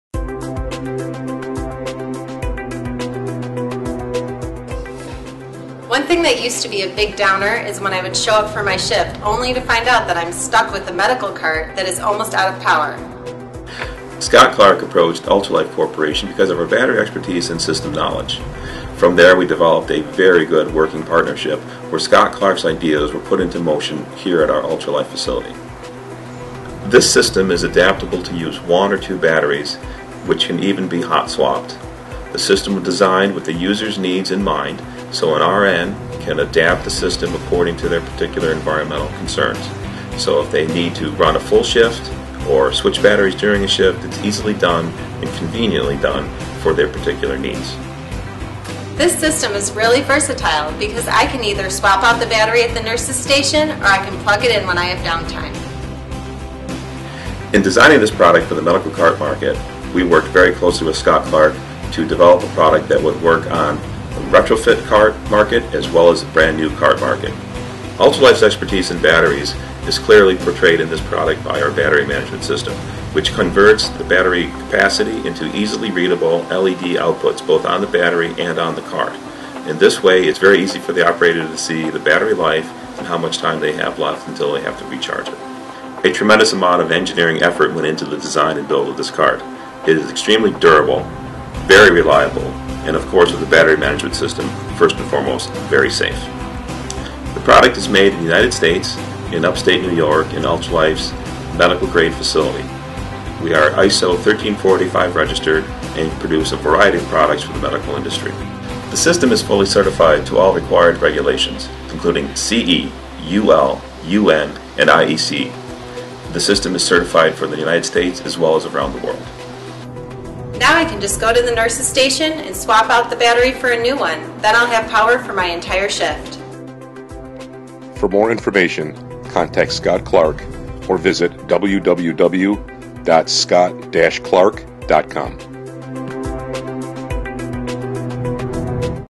Medical Cart Battery & Power System
Ultralife & Scott-Clark Medical jointly developed this Medical Cart Battery and Power Supply System with hot swap capabilities - an intelligent power supply system...